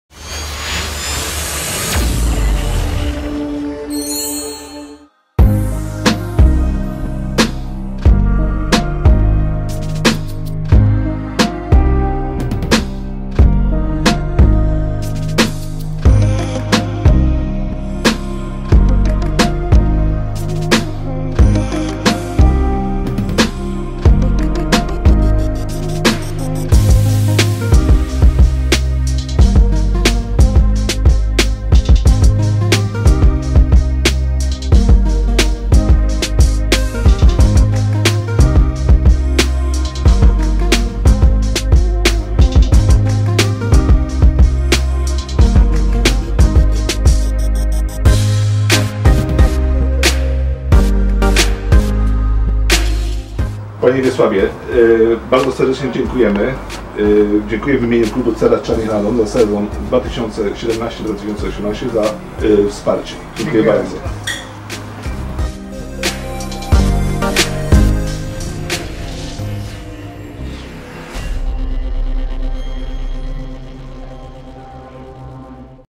CzarniTV: Z podziękowaniami w firmie Kobza Narzędzia
Kolejnym naszym przystankiem w ramach podziękowań za wsparcie w sezonie 2017/2018 była firma Kobza narzędzia, prowadzona przez Pana Wiesława Kobzę. 🛠️🔧🏐